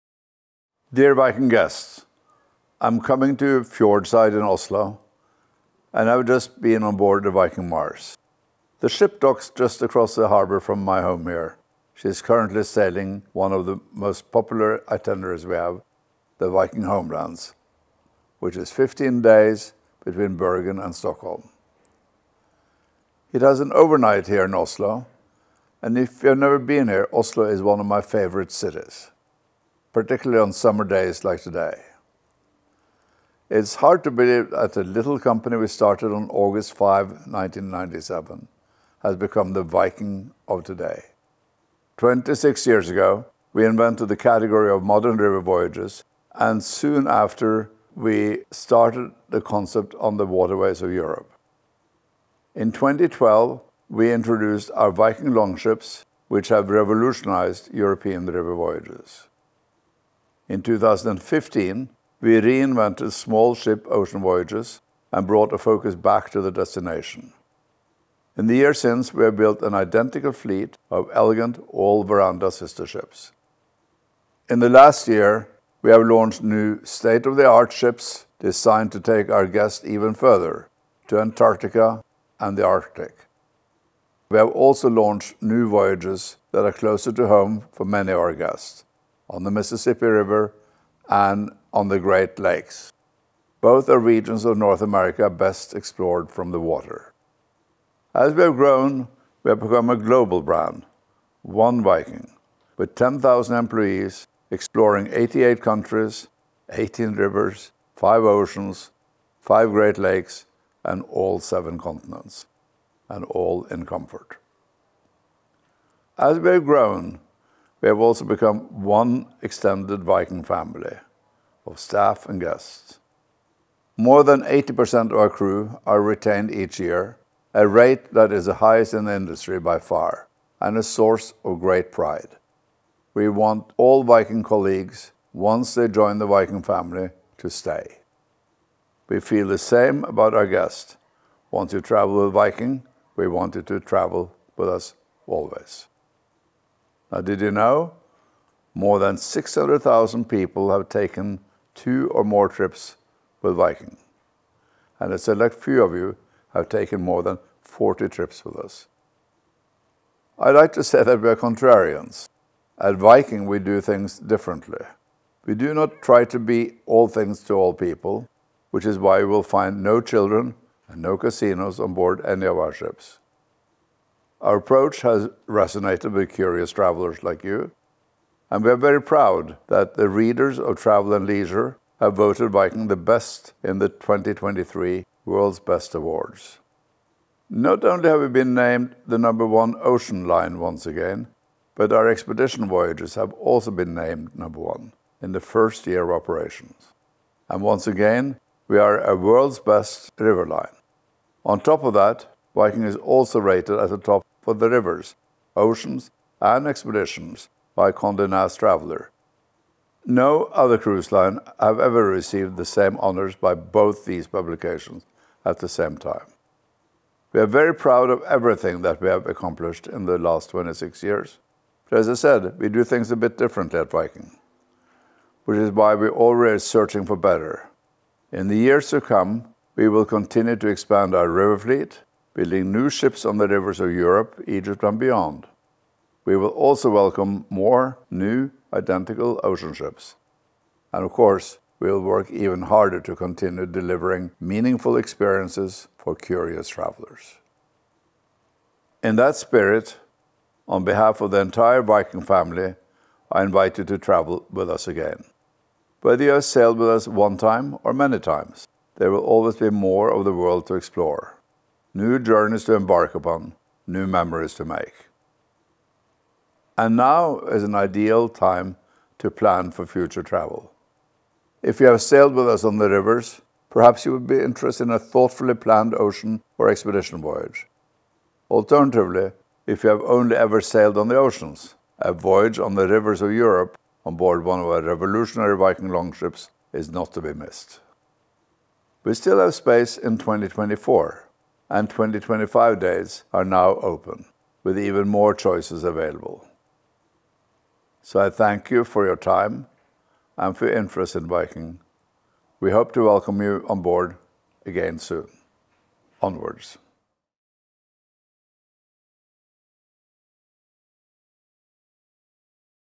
Celebrating Viking's 26th Anniversary
To mark Viking's 26th anniversary, Chairman Torstein Hagen shares a few key stats about the loyalty the brand has established with its extended Viking family of guests and employees:

• 10,000 employees with an 80% crew retention rate

• 600,000 guests have taken 2 or more trips with Viking

• A select few guests have taken more than 40 trips with Viking

In addition to the support of guests and employees, Viking's destination-focused approach has helped make it the world’s leading exploration company. Learn more about Viking's offerings on all 7 continents, as well as the historic recognition it received from the readers of Travel + Leisure and Condé Nast Traveler